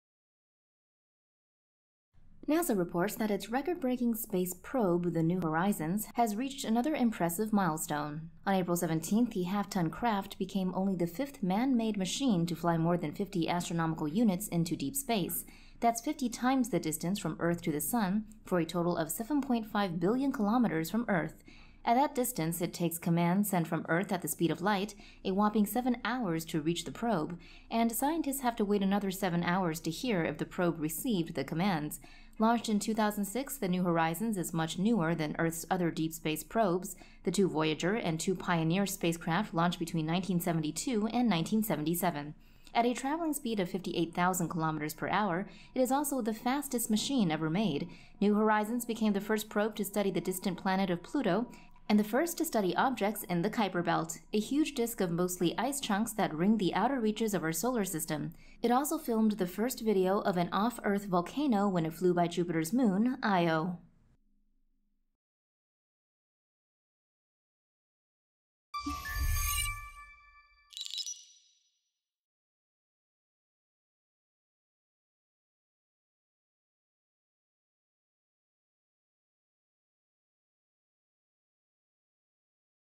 Earth's newest deep-space probe reaches edge of solar system
The New Horizons spacecraft has reached 50 astronomical units; a distance 50 times the distance from Earth to the Sun, or a total of 7.5 billion kilometers from Earth.

At that distance, it takes commands sent from Earth at the speed of light, a whopping 7 hours to reach the probe; and scientists have to wait another seven hours to hear if the probe received the commands.

RUNDOWN SHOWS:
1. Show space probe New Horizons (NH) flying from left to right into deep space
2. Show distance between sun and Earth, show 50 times that distance from Earth to NH
3. Show comms antenna pulsing light beam to NH, then NH pulsing light beam back
4. Show NH and its launch date, 2006, next to Earth's other probes and their launch dates
5. NH passing in front of an outer planet and its moon
6. NH flying through Kuiper Belt, zoom in to show giant comparative size of belt's ice chunks

VOICEOVER (in English):
NASA reports that its record-breaking space probe, the New Horizons, has reached another impressive milestone.

On 17 April the half-ton craft became only the fifth man-made machine to fly more than 50 astronomical units into deep space.

That's 50 times the distance from Earth to the Sun, for a total of 7.5 billion kilometers from Earth.

Launched in 2006, the New Horizons is much newer than Earth's other deep-space probes, the two Voyager and two Pioneer spacecraft launched between 1972 and 1977.

At a travelling speed of 58,000 kilometers per hour, it is also the fastest machine ever made.

New Horizons became the first probe to study the distant planet of Pluto, and the first to study objects in the Kuiper Belt — a huge disk of mostly ice chunks that ring the outer reaches of our solar system.

It also filmed the first video of an off-Earth volcano when it flew by Jupiter's moon, Io.